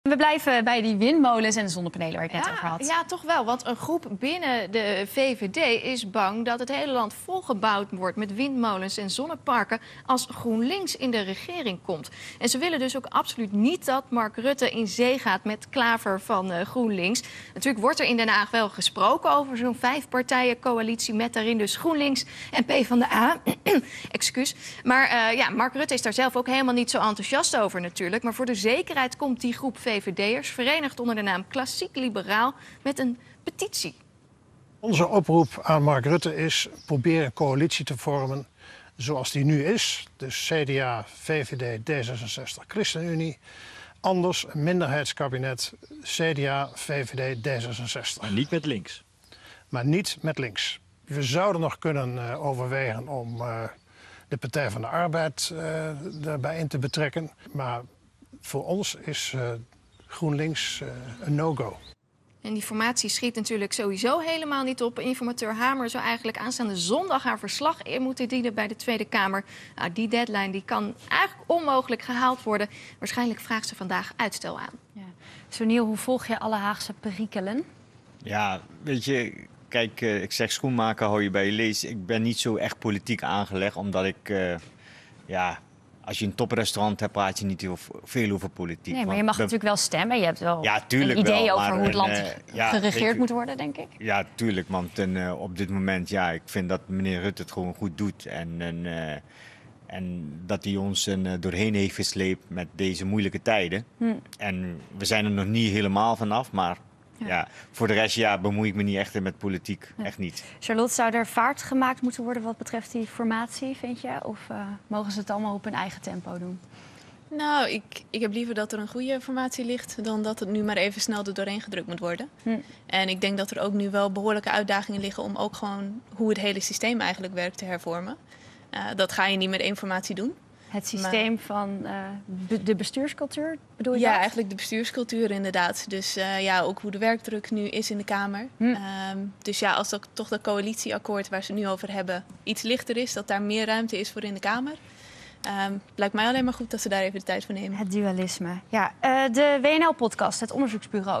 Groep VVD'ers wil absoluut niet dat partij in zee gaat met GroenLinks
Een groep binnen de VVD is bang dat het hele land volgeplempt wordt met windmolens en zonneparken als GroenLinks in de regering komt. Ze willen dan ook absoluut niet dat VVD-leider Mark Rutte in zee gaat met Jesse Klaver. De groep, verenigd onder de naam 'Klassiek Liberaal', komt daarom met een petitie.

Maak nu het verschil. Het telt. Word lid.